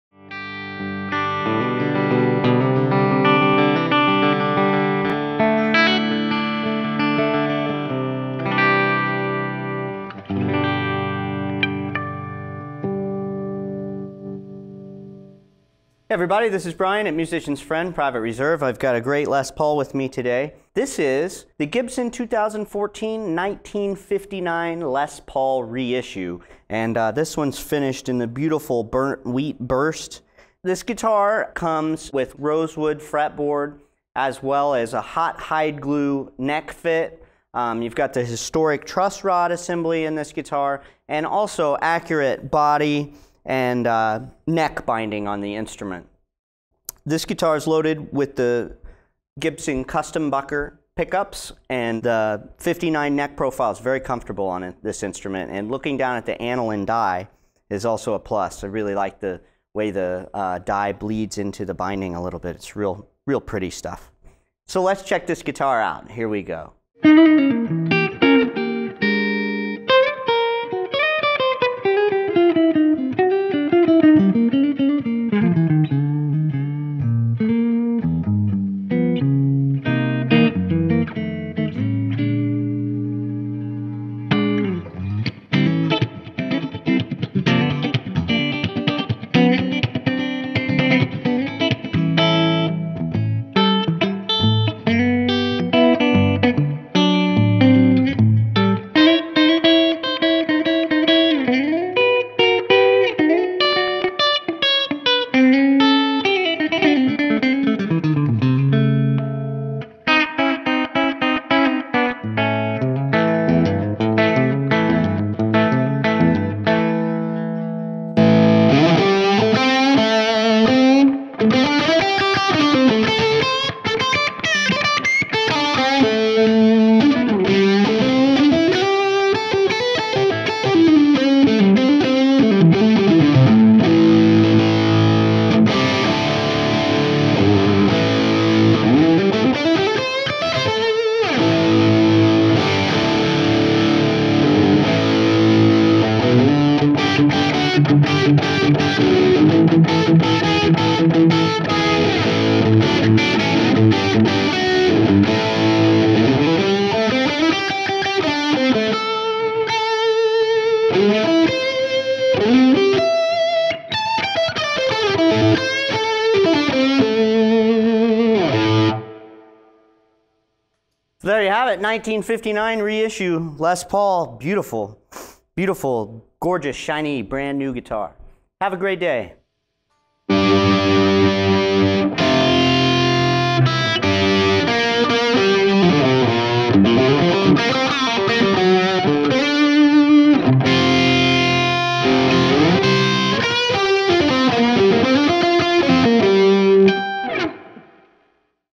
Gibson 2014 1959 Les Paul Reissue Electric Guitar, Burnt Wheat Burst
This guitar was played out of the box with factory strings, no additional set-up and no audio post production.

Serial# 942826, Weight:  8.86 lbs.

Signal chain used on this guitar:

Cables: Mogami

Pedals: {Amp effects loop: Boss DD-7 Digital Delay → → Boss RV-5 Digital Reverb}

Amp: Friedman Brown Eye 100W 2-Channel Tube Guitar Head

Mics: Shure SM57 & Neumann U87Ai

Recorded at Musician’s Friend in September 2015.